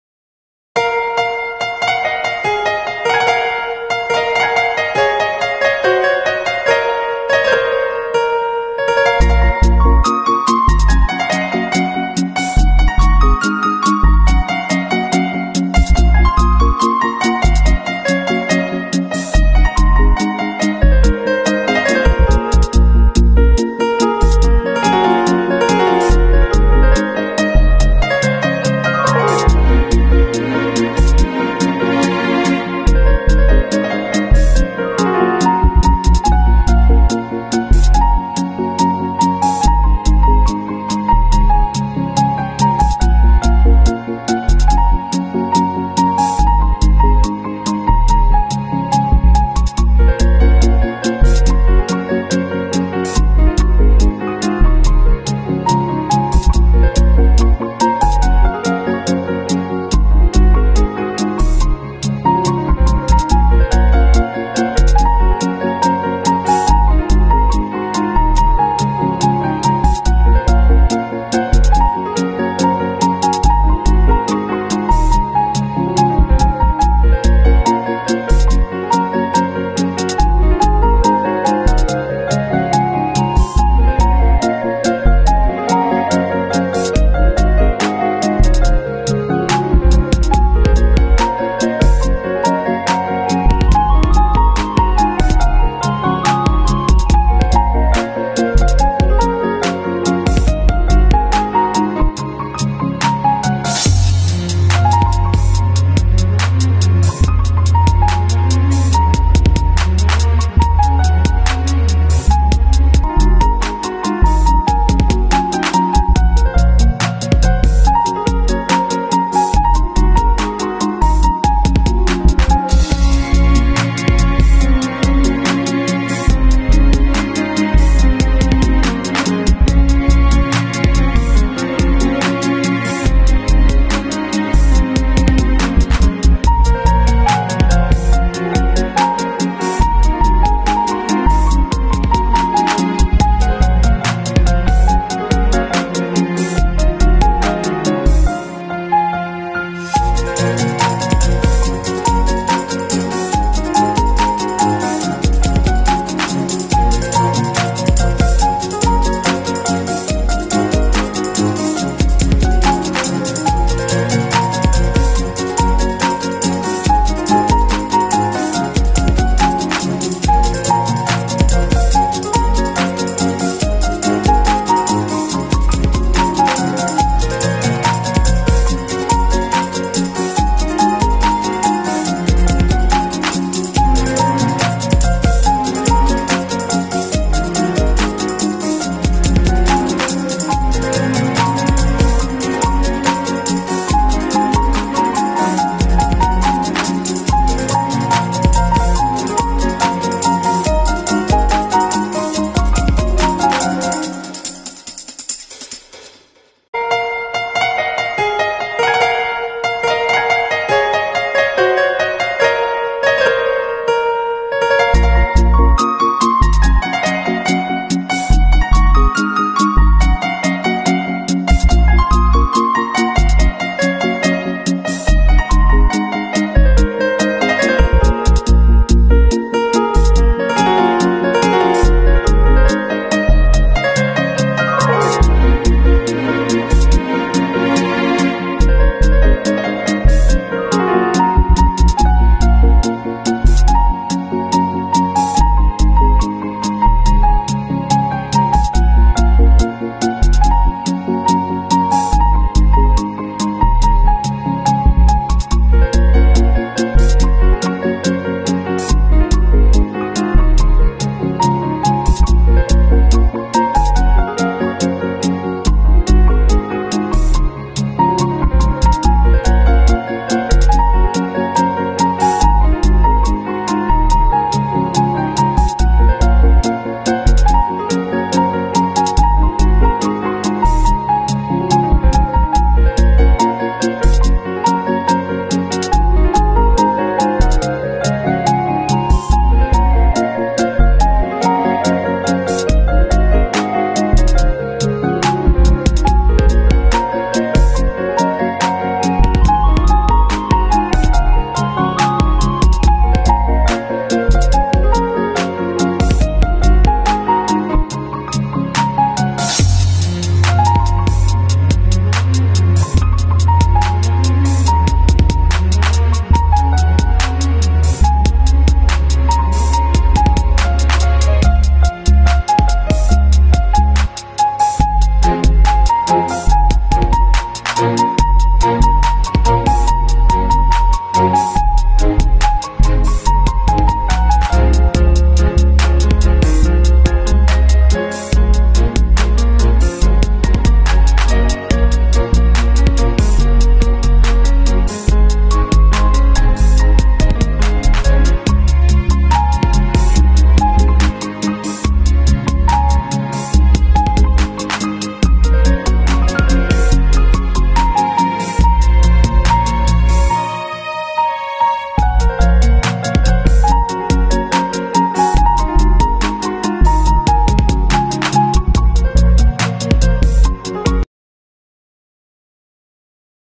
Mix Relaxing Piano with Joyful Dancing Flowers
Music increases the positive energy.Reduce stress being in nature, or even viewing scenes of nature, reduces anger, fear, and stress and increases pleasant feelings. Exposure to nature not only makes you feel better emotionally, it contributes to your physical wellbeing, reducing blood pressure, heart rate, muscle tension, and the production of stress hormone